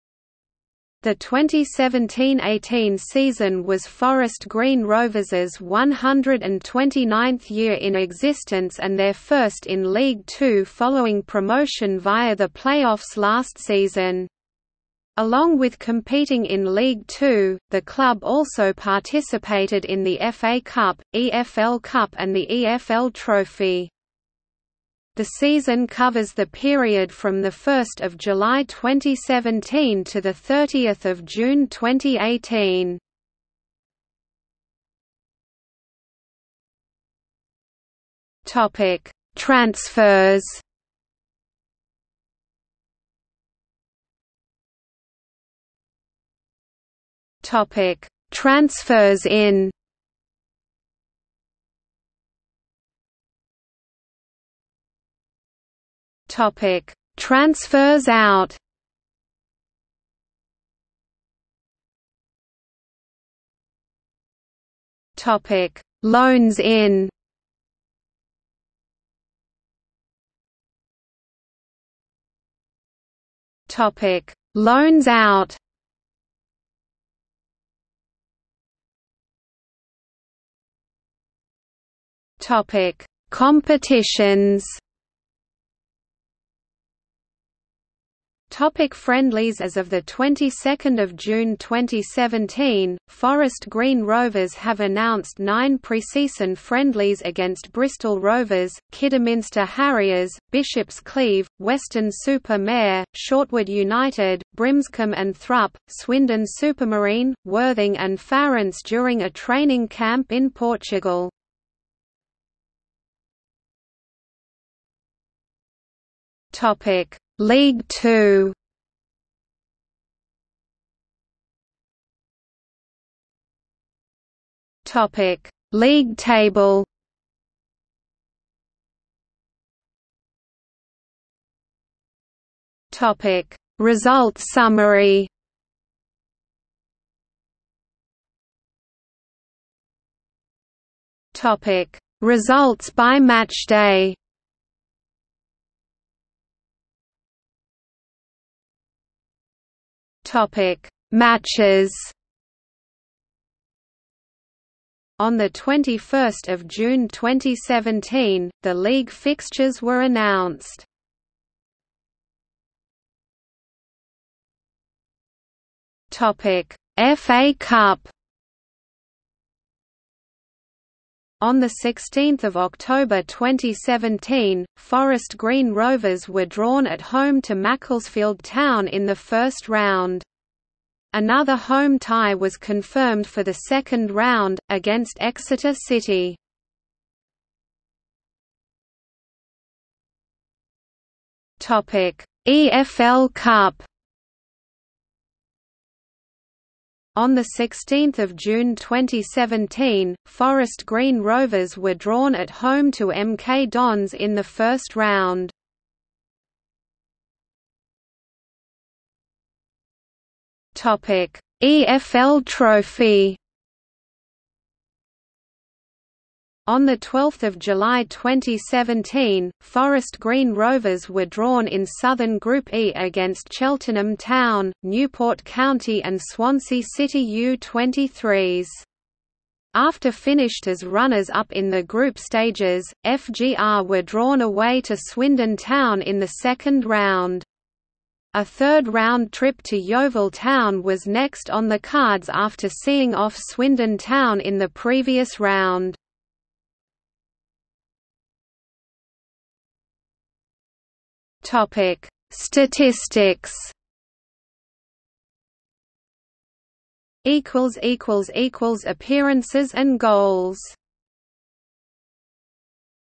2017–18 Forest Green Rovers F.C. season | Wikipedia audio article
00:00:35 1 Transfers
00:00:44 1.1 Transfers in
00:00:53 1.2 Transfers out
00:01:01 1.3 Loans in
00:01:10 1.4 Loans out
00:01:18 2 Competitions
00:01:28 2.1 Friendlies
00:01:53 2.2 League Two
00:02:01 2.2.1 League table
00:02:09 2.2.2 Result summary
00:02:18 2.2.3 Results by matchday
00:02:27 2.2.4 Matches
00:02:42 2.3 FA Cup
00:03:08 2.4 EFL Cup
00:03:27 2.5 EFL Trophy
00:04:07 3 Statistics

- Socrates

SUMMARY
The 2017–18 season was Forest Green Rovers's 129th year in existence and their first in League Two following promotion via the play-offs last season. Along with competing in League Two, the club also participated in the FA Cup, EFL Cup and the EFL Trophy.
The season covers the period from 1 July 2017 to 30 June 2018.